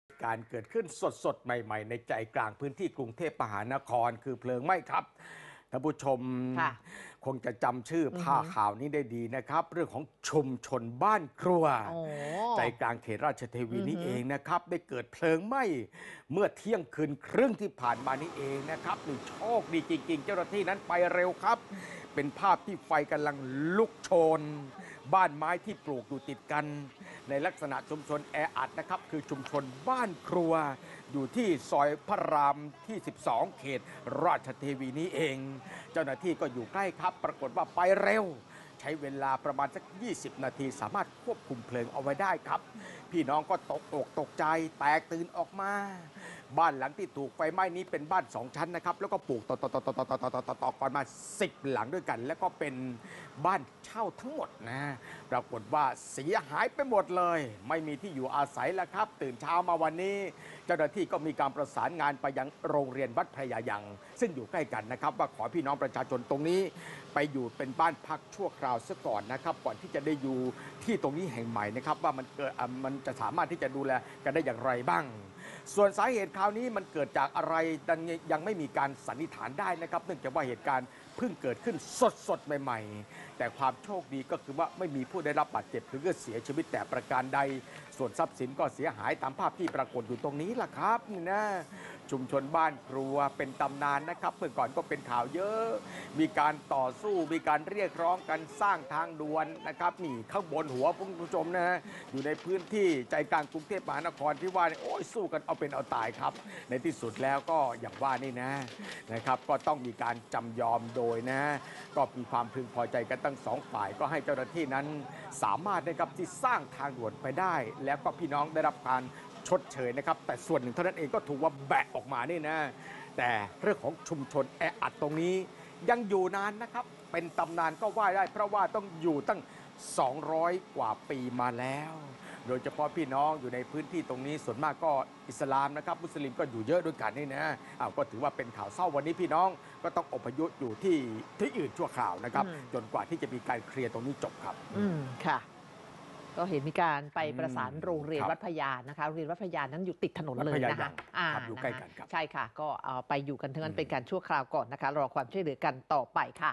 ไฟไหม้ชุมชนแออัดซอยพระรามหก ไร้เจ็บ
ไฟไหม้ชุมชนบ้านครัวใต้ ซอยพระรามหก ที่ 12 เขตราชเทวี กลางดึก เจ้าหน้าที่คุมเพลิงได้แล้ว บ้านไม้ 2 ชั้น แบ่งห้องเช่า วอด ตำรวจประสานโรงเรียนวัดพระยายัง เป็นสถานที่พักอาศัยชั่วคราวให้ชาวบ้านที่เดือดร้อน

1.สารคดีเฉลิมพระเกียรติ
2.พระราชกรณียกิจ-พระมหากรุณาธิคุณ
3.คนไทยหัวใจ♥รักในหลวง